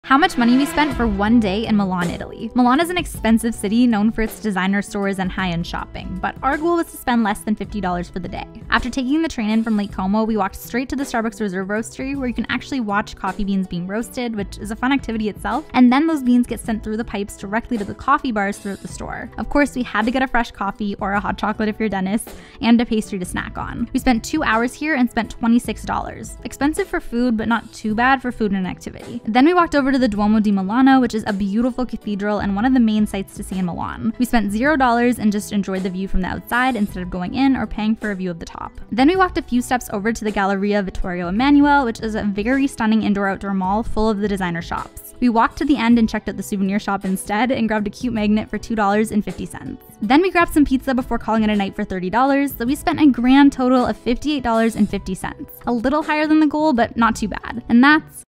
How much money we spent for one day in Milan, Italy 👀
Milan is EXPENSIVE - at least, it can be! We were there for less than 24 hours, but we saw so many high-end stores, luxury cars and people carrying designer bags. But our goal was to spend less than $50 - here’s how we did -

➡️ Starbucks Reserve Roastery ☕️🥐 what’s great about this spot is you can watch coffee beans being roasted - the get poured out, spun around, sucked back up through pipes, then pipes carry the beans around the store, to the bagging section at the back and directly to coffee bars throughout the store! We spent a few hours there watching the process and eating a snack. 💰COST = $26
➡️ Duomo di Milano ⛪️🦅 this is a big cathedral and one of the main sights to see in Milan! We stuck to enjoying a free view from the outside. It was super busy there, with both people and pigeons! 💰COST = $0
➡️ Galleria Vittorio Emanuele II 🛍️💸 this is a beautiful indoor / outdoor mall full of expensive stores (and exclusively expensive stores) - we went to the souvenir shop at the very end and picked up a cute magnet, instead. 💰 COST = $2.50
➡️ Napiz Milano 🍕🥂 last stop, dinner! The restaurants with good ratings are all pretty high cost (aka a minimum of €24 per dish), but we found a delish pizza spot for much less. 💰COST = $30

So we spent a grand total off… $58.50! 🤌🏿👏🏻

Remember, you can travel for cheaper - but you have to be selective and do your research. Any tips for spending less when you visit expensive cities? Let us know!